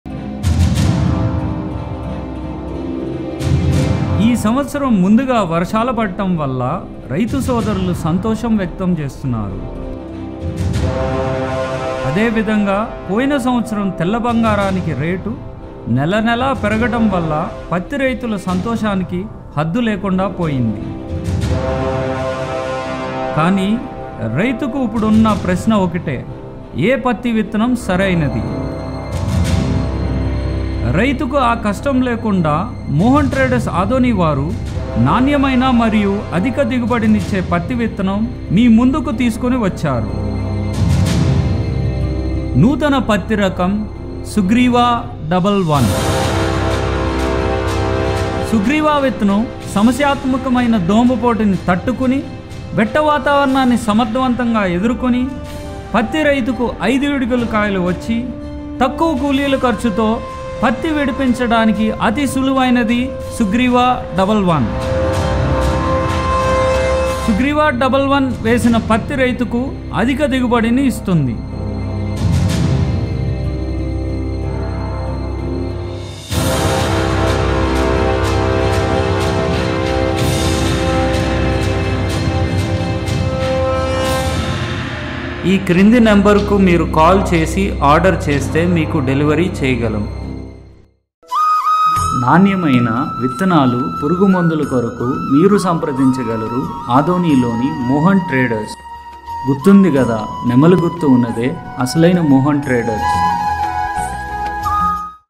ఏ పత్తి విత్తనం సరైనది..? - 9493636363 -మోహన్ ట్రేడర్స్ ఆదోని
మోహన్ ట్రేడర్స్ ఆదోని వారు గత 25 సంవత్సరాలుగా నాణ్యమైన పత్తి విత్తనాలు రైతులకు అందిస్తున్నారు. నూతన రకం సుగ్రీవ 11 ను ఈ సంవత్సరం రైతులకి పరిచయం చేస్తున్నారు.

రైతు సోదరులకు ముఖ్య గమనిక:- మీరు ఈ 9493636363 నెంబర్ కు కాల్ చేసి ఆర్డర్ చేస్తే మేము మీకు డెలివరీ చేయగలం - మోహన్ ట్రేడర్స్ ఆదోని